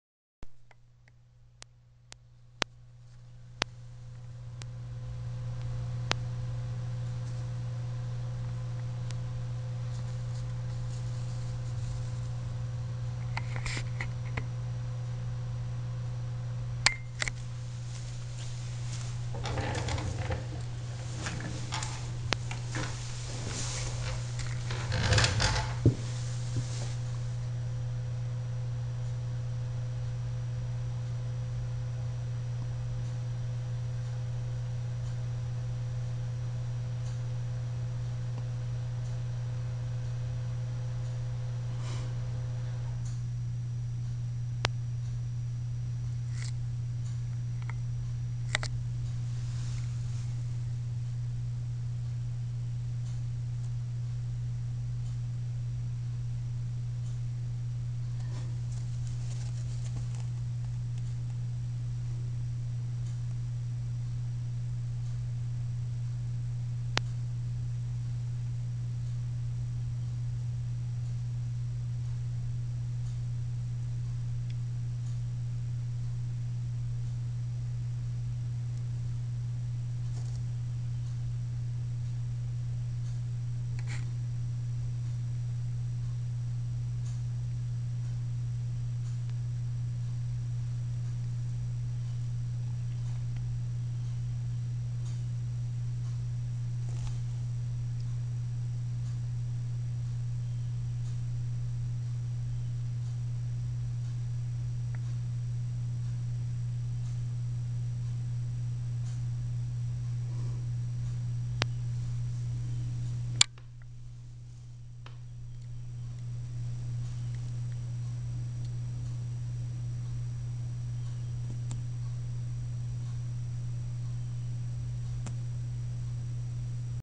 Kane and the Mulch
This is Kane after a trip to the vet where they sedated him to remove a piece of mulch that was wedged between his back teeth agianst the roof of his mouth.